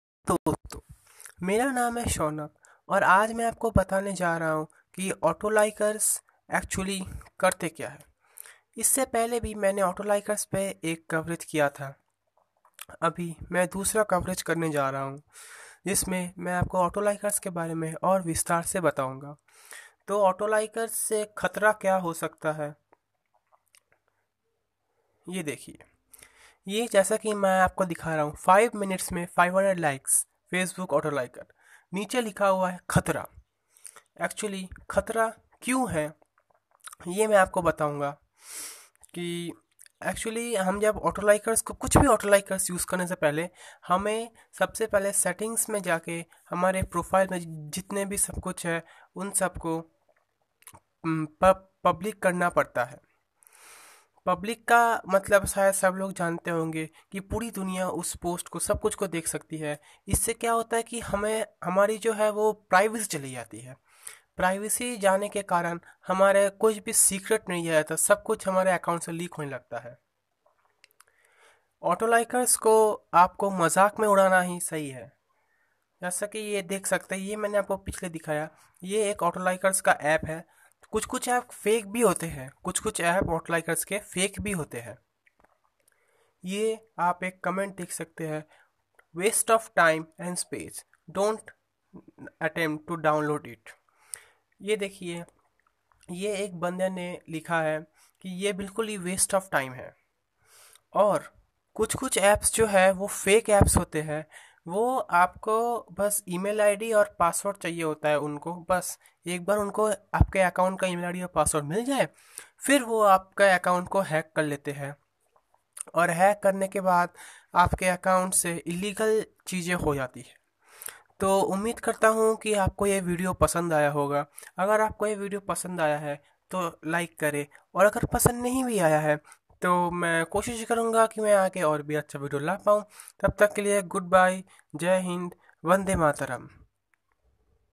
Autolikers! Don't Use It
Hello friends today I am going to tell you that you should not use the autolikers, this led to hack your account.